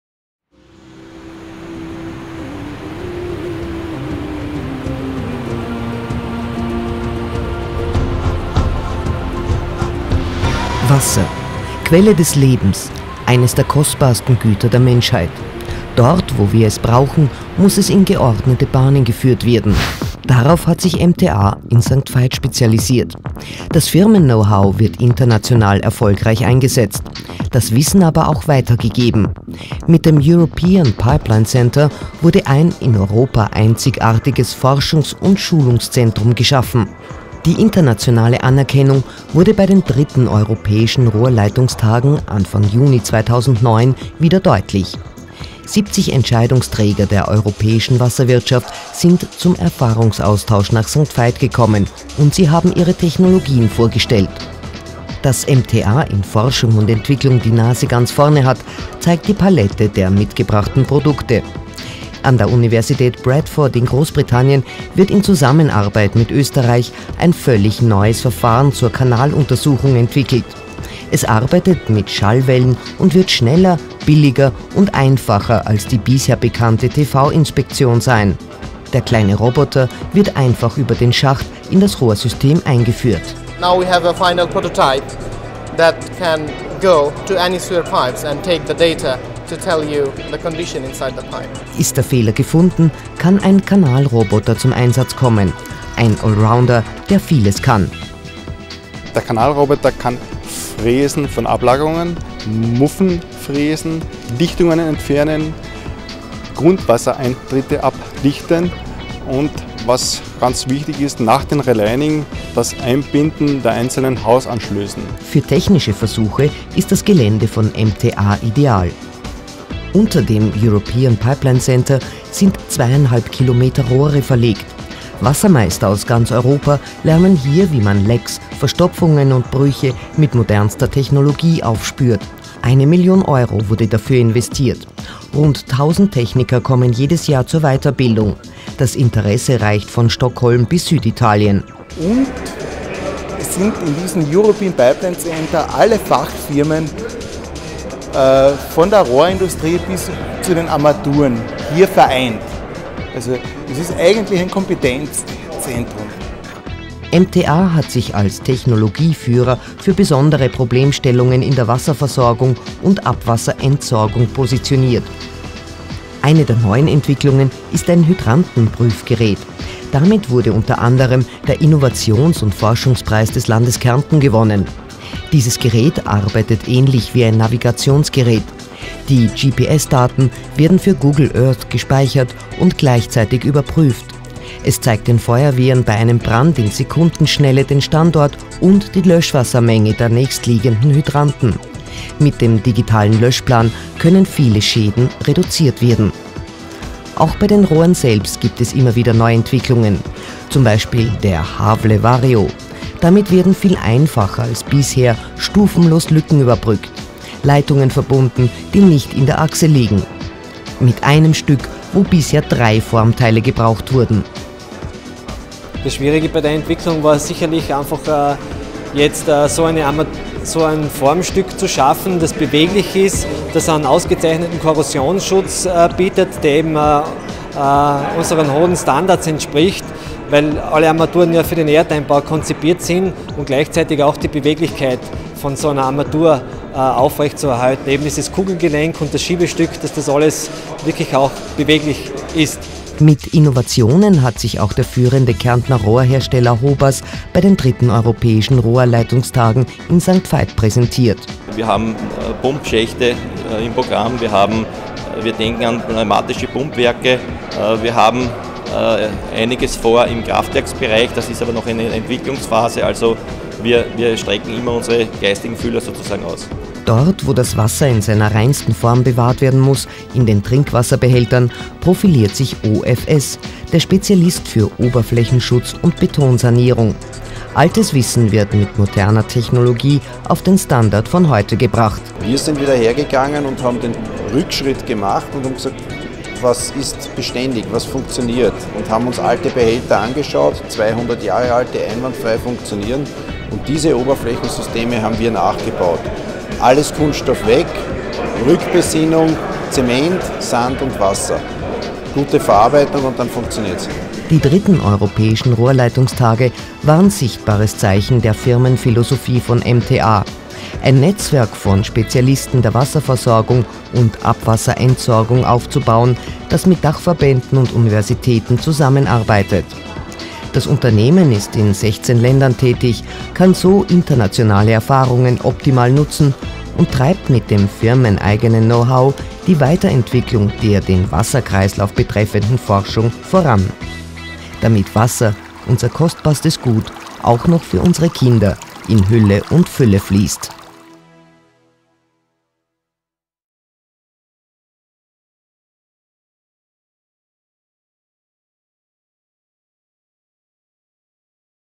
3. Europäische Rohrleitungstage 2009
Rückblick auf die 3. Europäischen Rohrleitungstage 2009 in St. Veit an der Glan.
Bei diesem Event präsentieren sich Europäische Hersteller und Dienstleister der Trink- und Abwasserbranche und zeigen die Ergebnisse ihrer Forschungs- und Zusammenarbeit in der Praxis.

Namhafte Europäische Vortragende aus Wissenschaft, Forschung und Wirtschaft informieren über aktuelle Technologien, Herausforderungen und Lösungen in der Trinkwasserversorgung, im Hochwasserschutz und der Abwasserentsorgung.

Die umfangreiche Fachausstellung wird begleitet von einem dichten Programm an Vorführungen, die den praktischen Einsatz der gezeigten Produkte und Services demonstrieren.
Die 4. Europäischen Rohrleitungstage finden von 29.-30. Juni 2011 statt.